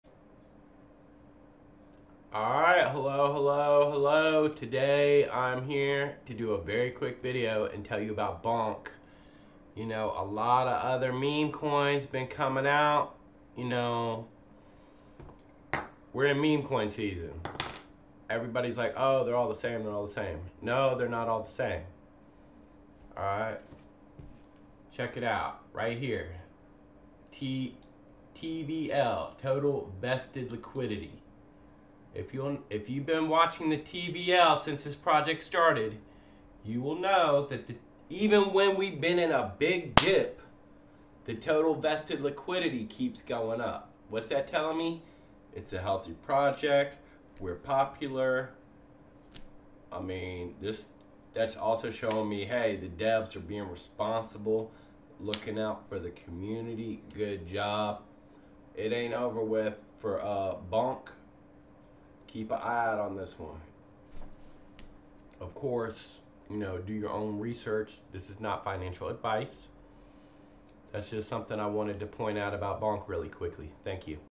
Bonk the new solana meme coin and the TVL, just one reason I'm bullish
What's separating bonk from the pack of the other new meme coins on Solana right now.